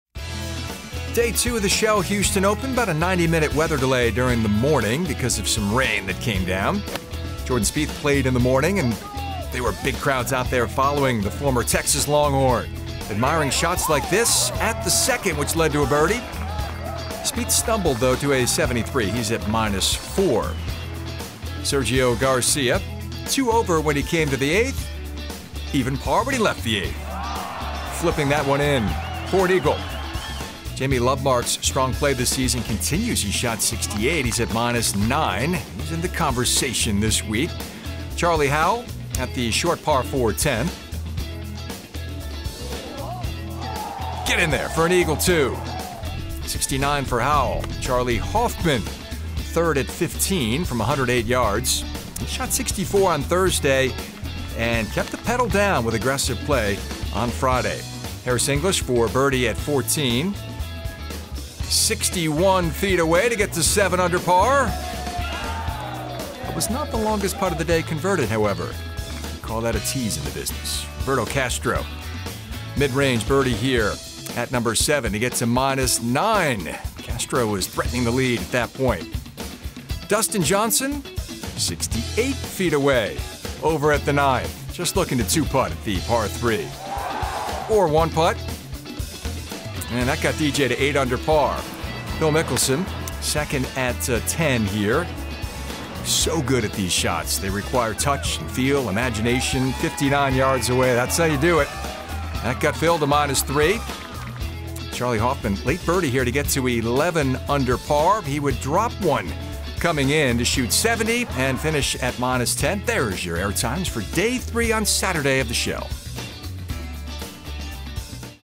Highlights | Charley Hoffman continues to lead at Shell Houston Open
In the second round of the 2016 Shell Houston Open, Charley Hoffman maintains a one-shot lead heading into a weekend with Jamie Lovemark, Dustin Johnson and Roberto Castro trails close behind. .